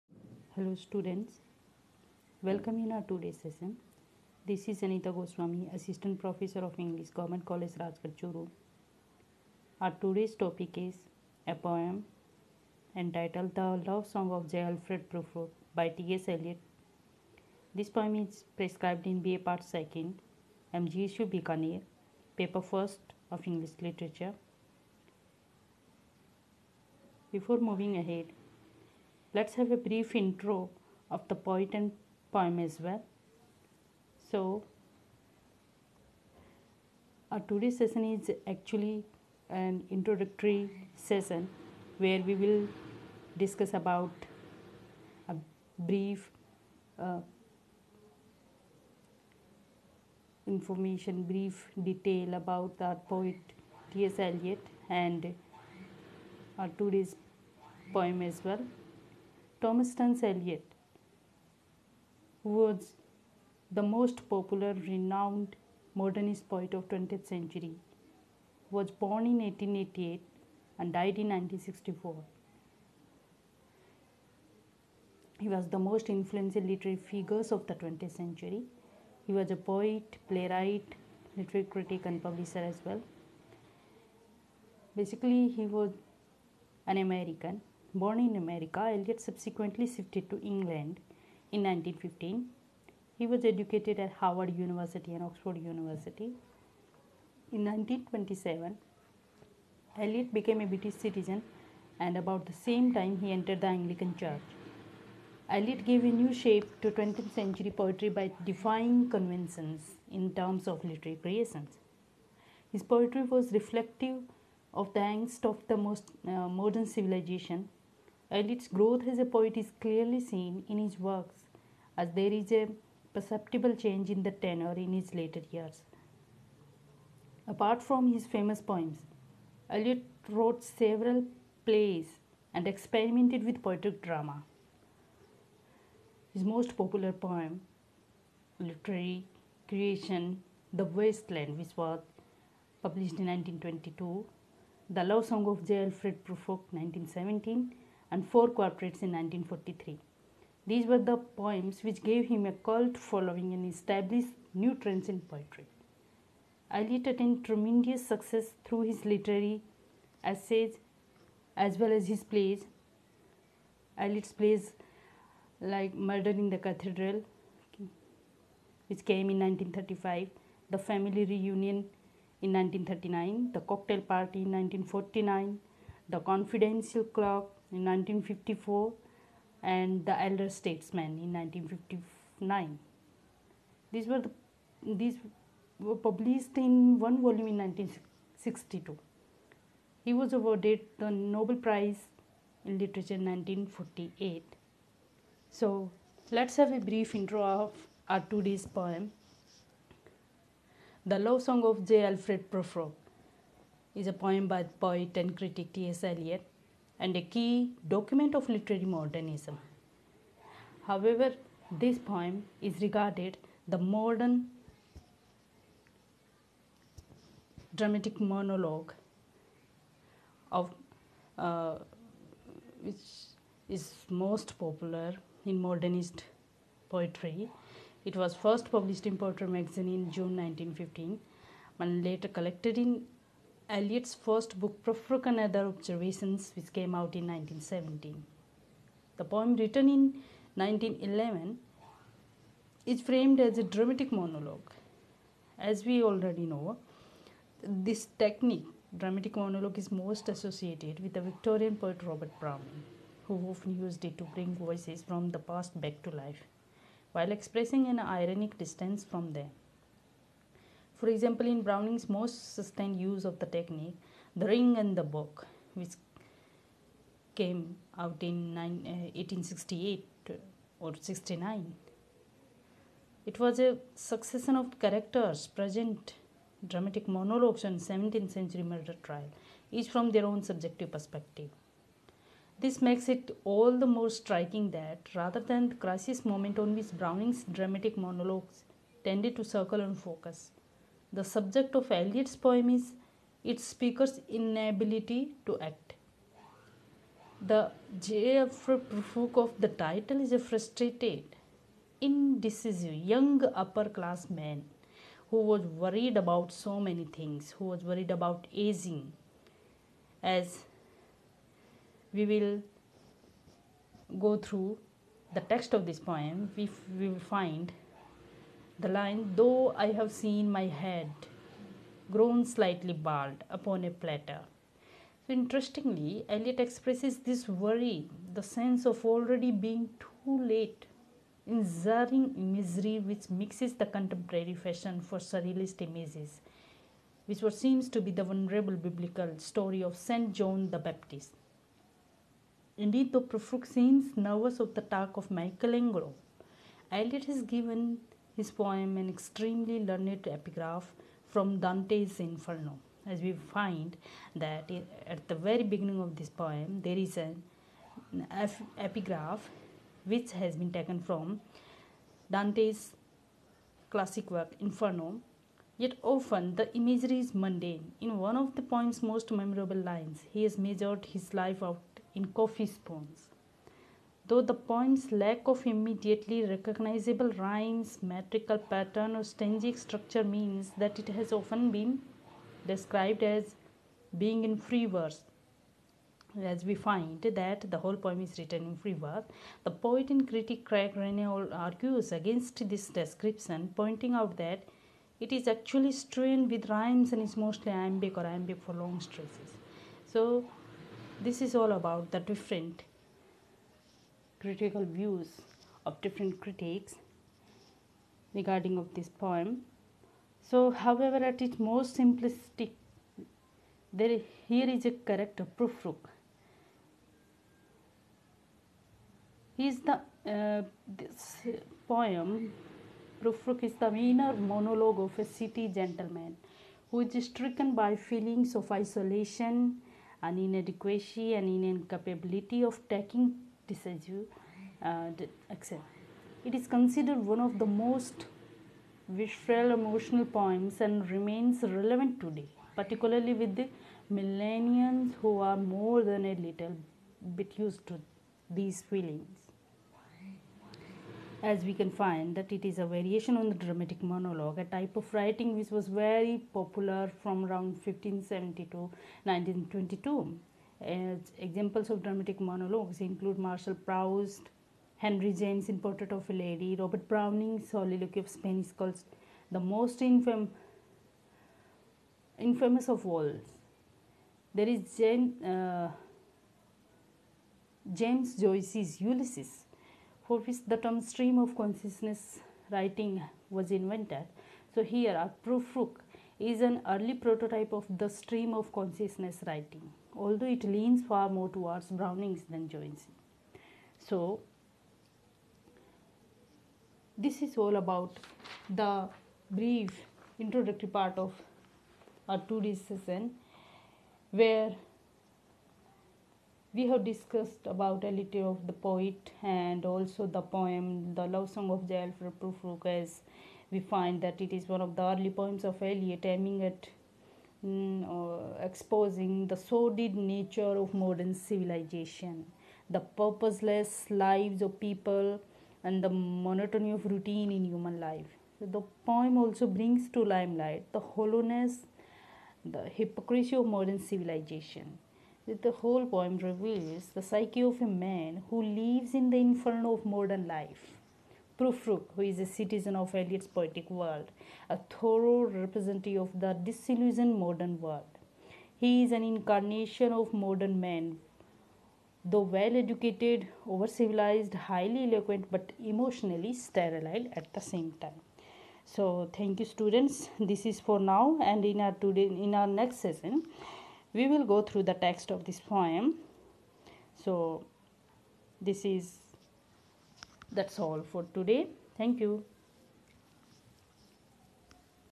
Introductory Part of the Poem The Love song of J Alfred Prufrock by T. S. Eliot
Anita Goswami, Assistant Professor of English GCRChuru